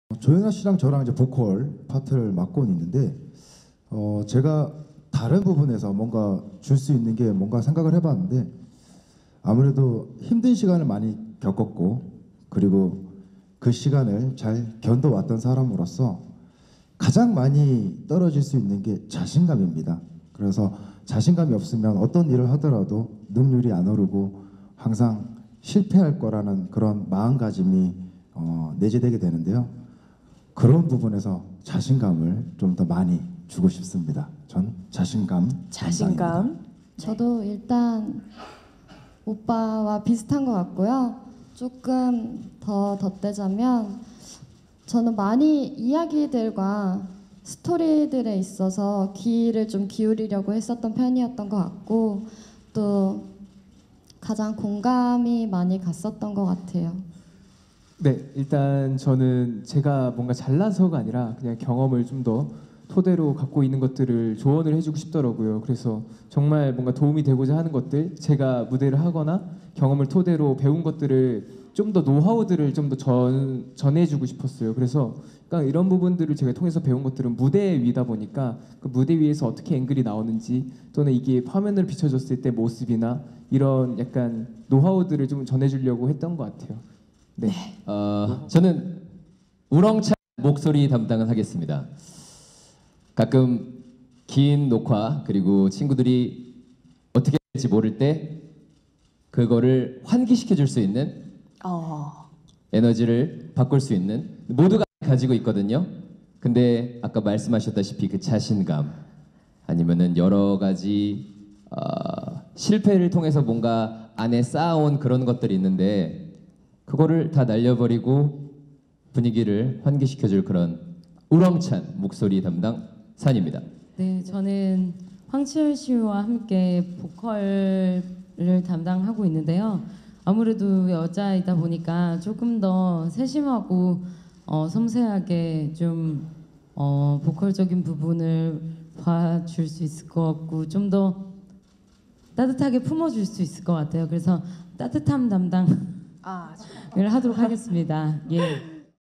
'더 유닛(THE UNIT)' 황치열(Hwang Chi yeul) "멤버들에게 자신감 주고파" [MD동영상]
'더 유닛(THE UNIT)' 황치열(Hwang Chiyeol) "멤버들에게 자신감 주고파" [MD동영상]

25일 오후 서울 영등포구 영등포동 타임스퀘어 아모리스홀에서 진행된 KBS 2TV 아이돌 리부팅 프로젝트 '더 유닛' 제작발표회에 참석한 가수 황치열이 프로그램을 함께하는 멘토로서 멤버들에게 자신감을 주고 싶다고 말했다.

'더 유닛'은 연예계 데뷔 경력이 있고 무대 위에서 꿈을 펼치고 싶은 참가자들의 재능, 잠재력을 발굴하고 대한민국을 대표하는 유닛 그룹을 만드는 프로그램이다. 최종적으로 남자 유닛그룹 9명, 여자 유닛그룹 9명을 선발하며 두 팀이 각각 활동한다.

'더 유닛'은 오는 28일 오후 9시 15분에 첫 방송된다.